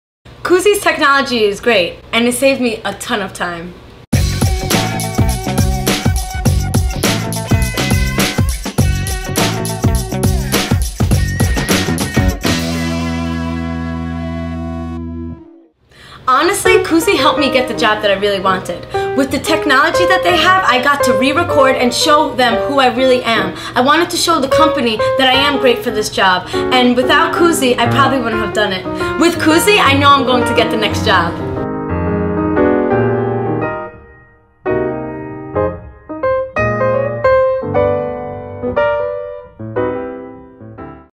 A Job Seeker's Testimonial
Ashely gives us a few thoughts about her experience with taking a video assessment through Cuzie's (One-way) interview system.